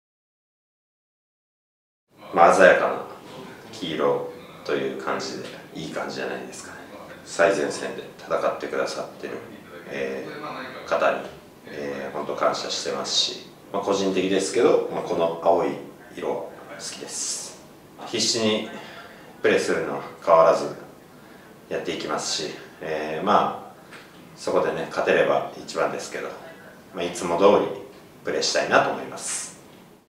ホークス公式 鷹の祭典2020 HawksInformation Mini 9柳田悠岐外野手
鷹の祭典2020 ホークスインフォメーション Mini
9柳田悠岐外野手からのメッセージ